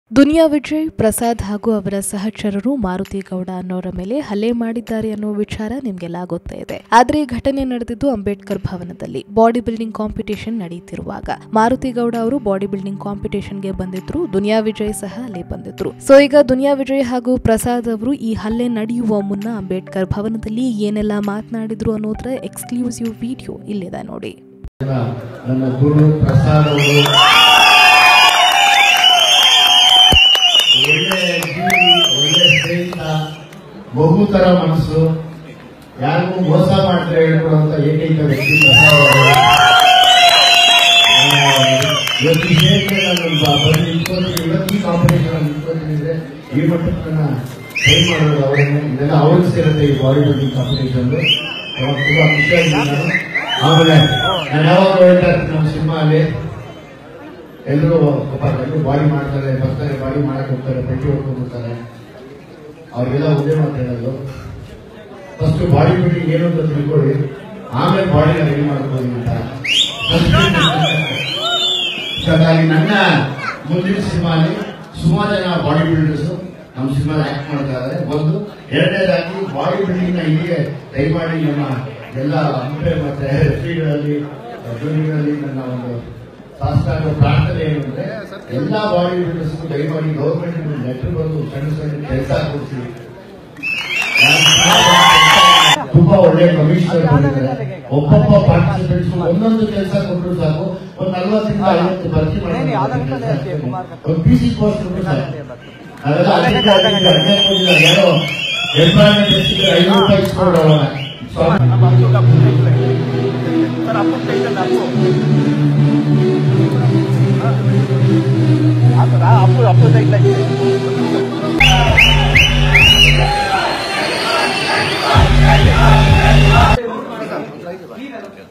ಅಂಬೇಡ್ಕರ್ ಭವನದಲ್ಲಿ ವಿಜಿ ಮಾತುಗಳು..! | Filmibeat Kannada
Duniya Vijay and Team gets arrested on Assaulting and Abduction charges. Here is an exclusive video of Duniya Vijay speaking in Ambedkar Bhavan before the incident.

 ಮಾರುತಿ ಗೌಡ ಅವರ ಮೇಲೆ ಹಲ್ಲೆ ಮಾಡುವ ಮುನ್ನ ದುನಿಯಾ ವಿಜಯ್ ಹಾಗೂ ಪ್ರಸಾದ್ ಅಂಬೇಡ್ಕರ್ ಭವನದಲ್ಲಿ ಮಾತನಾಡಿದ ವೀಡಿಯೋ ಇದು.